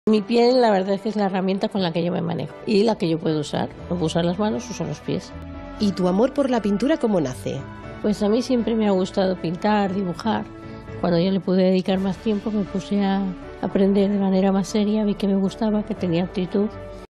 Entrevista a nuestra artista con el pie Virginia Calderón
Nuestra artista Virginia Calderón ¡cumple 20 años en nuestra asociación! Una parálisis permanente de ambos brazos debida a dificultades en el parto, le obligó a utilizar los pies, algo que no le detuvo para perseguir sus sueños. ¡Felicidades Virginia!